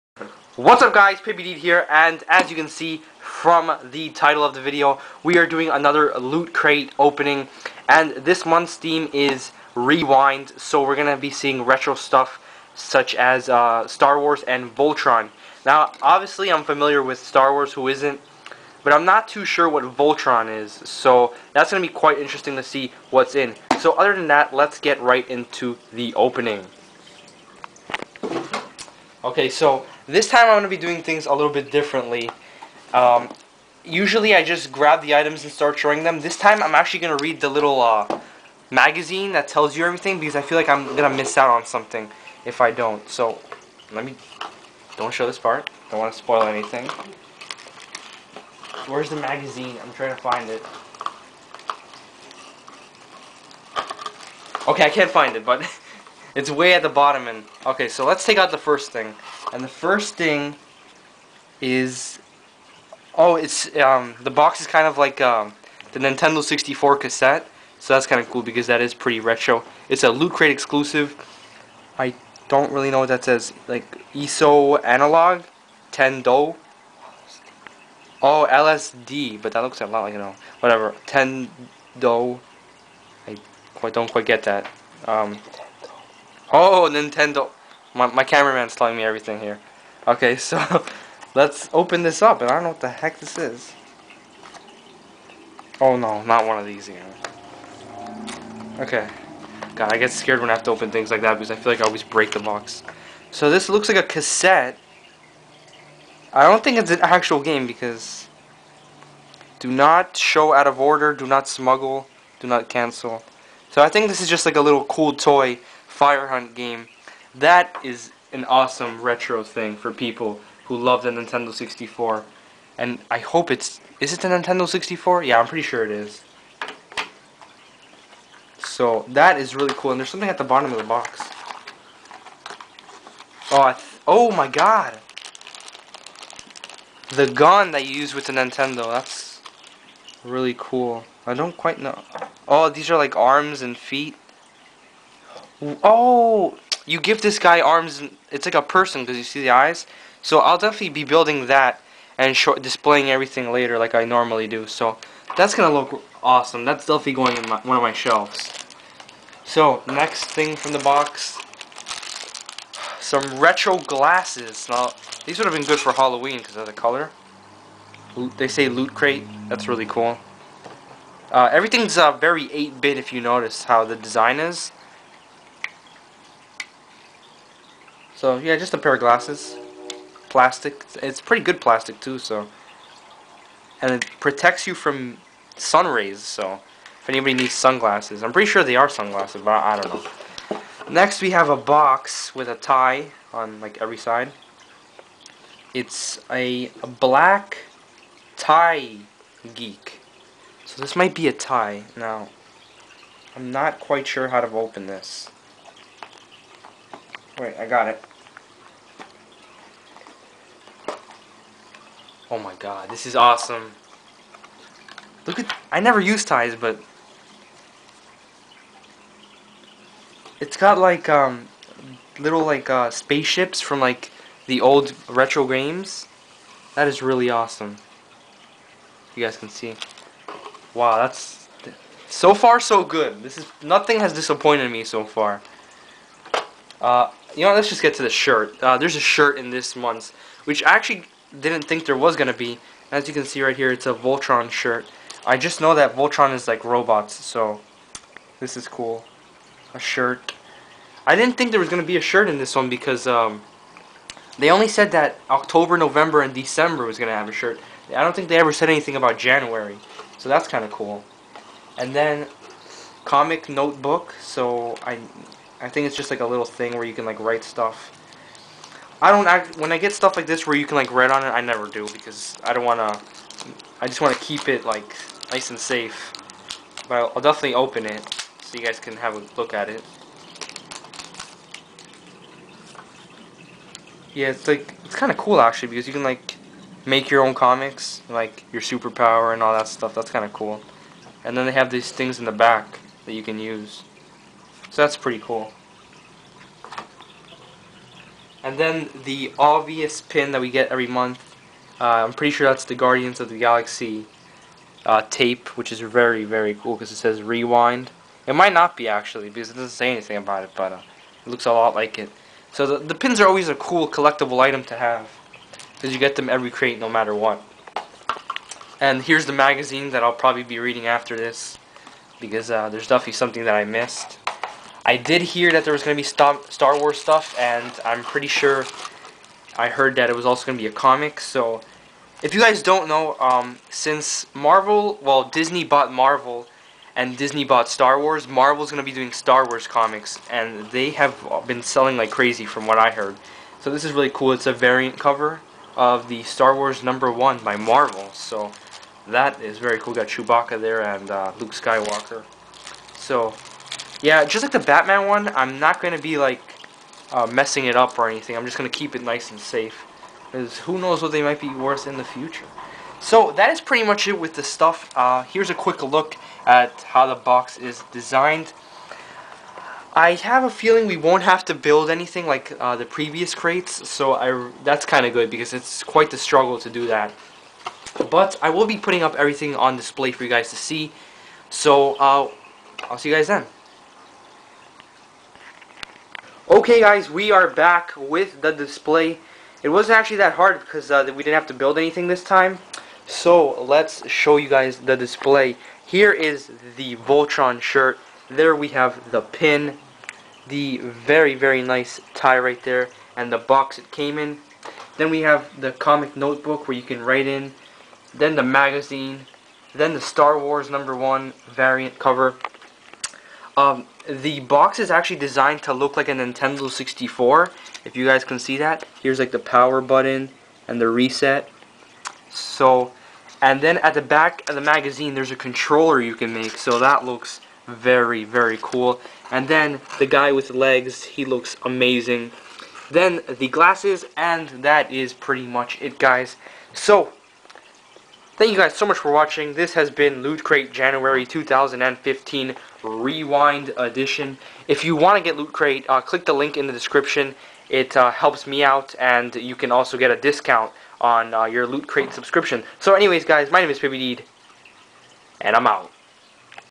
LootCrate - January 2015 - Rewind Edition
Today we open the LootCrate Rewind Edition! I was a bit skeptical about how good this crate might be and it ended up surprising me. I'll rate it a solid 8/10!